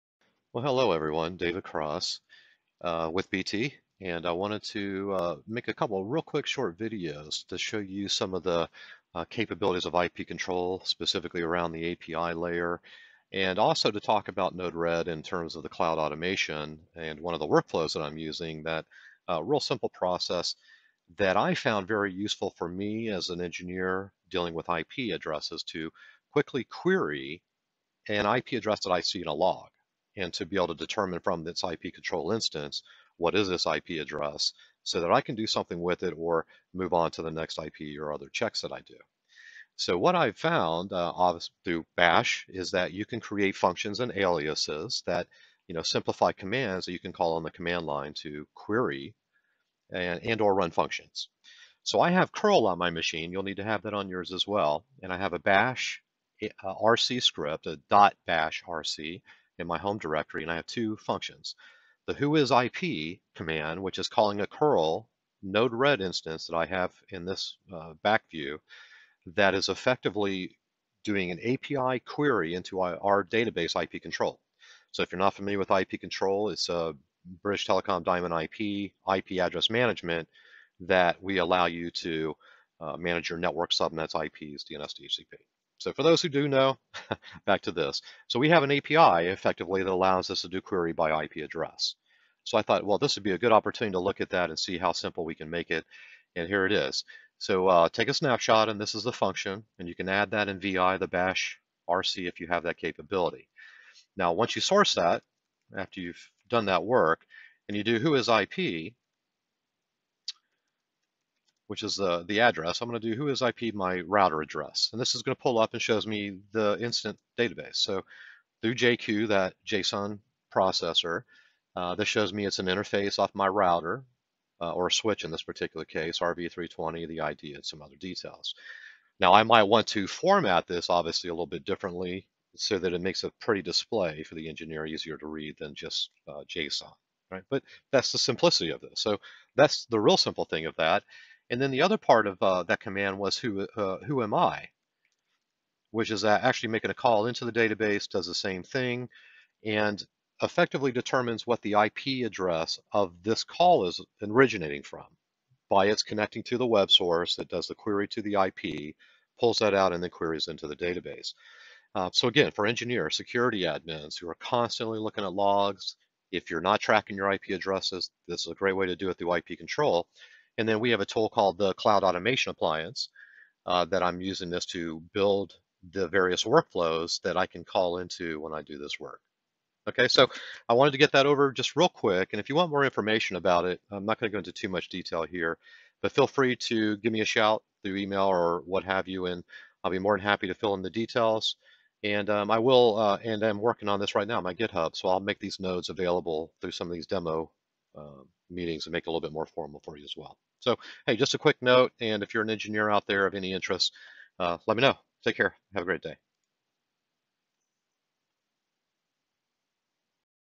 BT IPControl Quick Whois IP and Whoami (IP) API functions
This short video demonstrates a number of features available through BT Diamond IP and IPControl IPAM.   The APIs available allow us to quickly query an IP to determine its "known" source from the IPAM authority (IP Control).  It then demonstrates two "bash" alias functions that call curl into a Node-Red Workflow that performs a API lookup of the IP using a read only account.